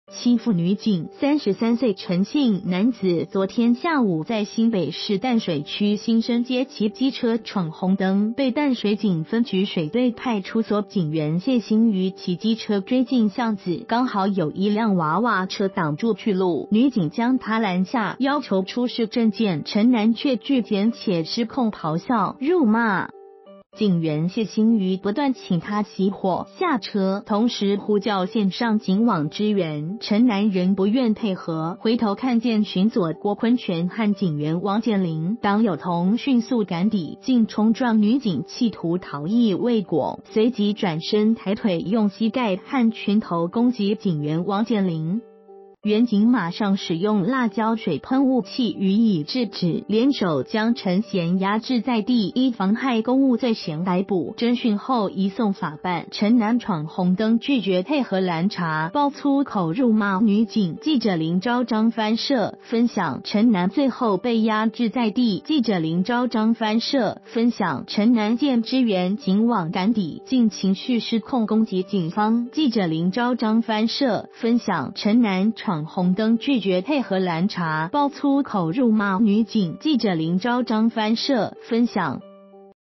闖紅燈不服女警攔查爆粗口 甚至揮拳出腳攻擊
發表評論評論如下。